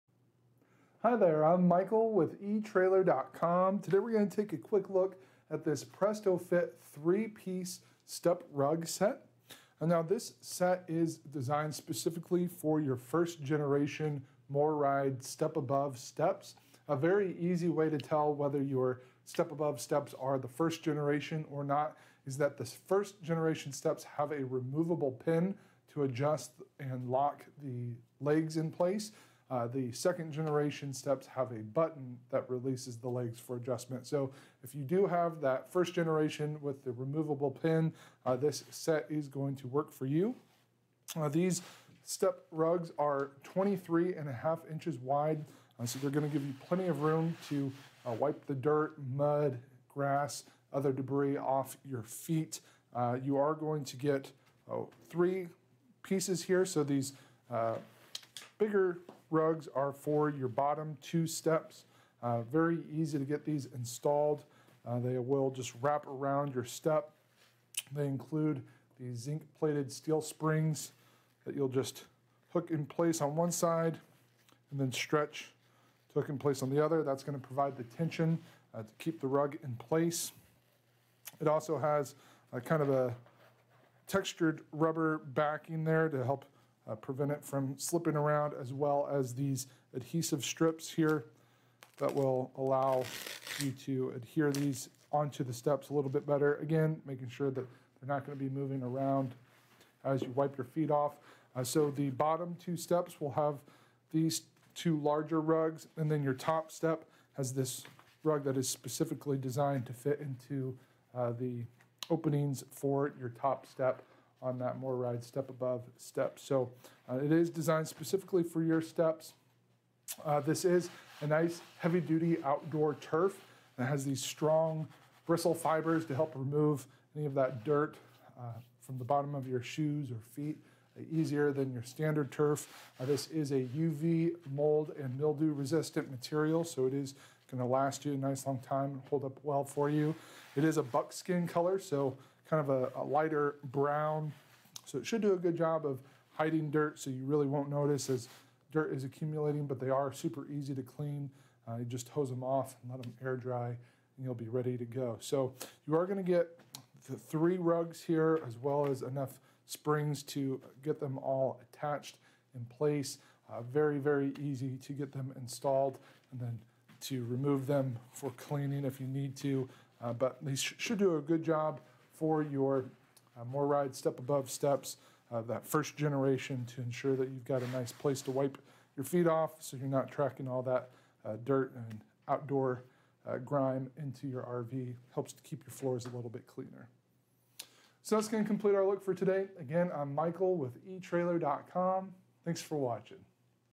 Review of Prest-O-Fit RV Step Covers - Buckskin Brown 3-Piece StepAbove Step Rug Set - PR45MR
Hi there, I'm Michael with etrailer.com. Today, we're gonna take a quick look at this Prest-O-Fit 3-piece Step Rug Set. Now, this set is designed specifically for your 1st Generation MORryde StepAbove Steps. A very easy way to tell whether your StepAbove Steps are the 1st Generation or not, is that these 1st Generation steps have a removable pin to adjust and lock the legs in place, the 2nd Generation steps have a button that releases the legs for adjustment. So if you do have that 1st Generation with the removable pin, this set is going to work for you. These step rugs are 23 1/2 inches wide, so it's gonna give you plenty of room to wipe the dirt, mud, grass, other debris off your feet.

You are going to get three pieces here, so these bigger rugs are for your bottom two steps, very easy to get these installed, they will just wrap around your step. They include the zinc-plated steel springs that you'll just hook in place on one side and then stretch, hook in place on the other, that's gonna provide a tension to keep the rug in place. It also has a kind of the textured rubber backing there to help prevent it from slipping around, as well as these adhesive strips here that will allow you to adhere these onto the steps a little bit better, again, making sure that they're not gonna be moving around as you wipe your feet off. So the bottom two steps will have these two larger rugs, and then your top step has this rug that is specifically designed to fit into the openings for your top step on that MORryde StepAbove Step, so it is designed specifically for your steps. This is a nice heavy-duty outdoor turf, it has these strong bristle fibers to help remove any of that dirt from the bottom of your shoes or feet, easier than your standard turf.

This is a UV, mold, and mildew resistant material, so it is gonna last you a nice long time, hold up well for you. It is a buckskin color, so kind of a lighter brown, so it should do a good job of hiding dirt, so you really won't notice as dirt is accumulating, but they are super easy to clean, just hose them off and let them air dry, and you'll be ready to go. So you are gonna get the three rugs here, as well as enough springs to get them all attached in place. Very, very easy to get them installed and then to remove them for cleaning if you need to. But these should do a good job for your MORryde StepAbove steps, that 1st Generation, to ensure that you've got a nice place to wipe your feet off, so you're not tracking all that dirt and outdoor grime into your RV, helps to keep your floors a little bit cleaner.